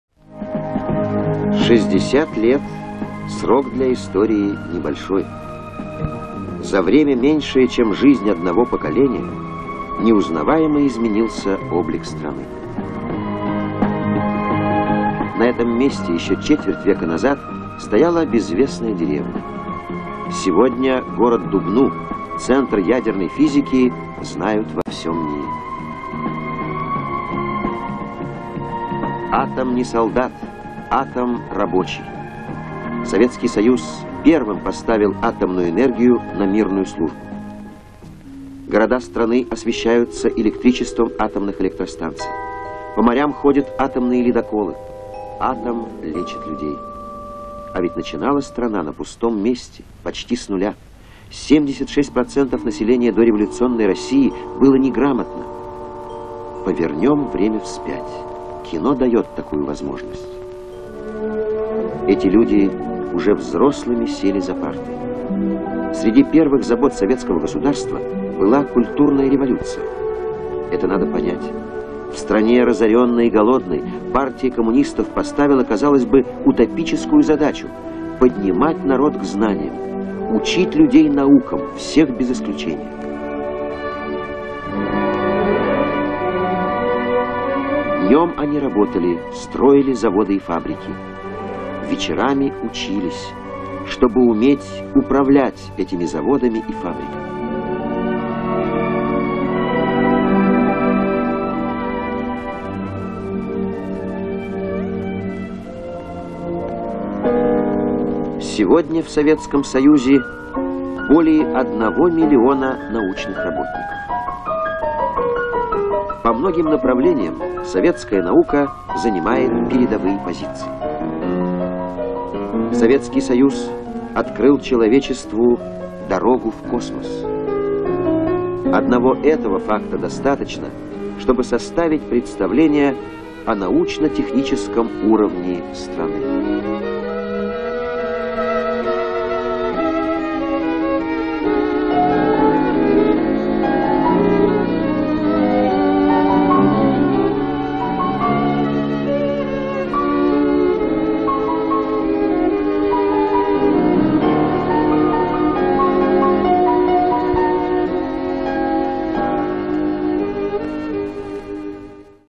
1976г. Дубна. Центр ядерной физики. Московская обл.
Знакомьтесь, Советский Союз. 1976г. ЦСДФ.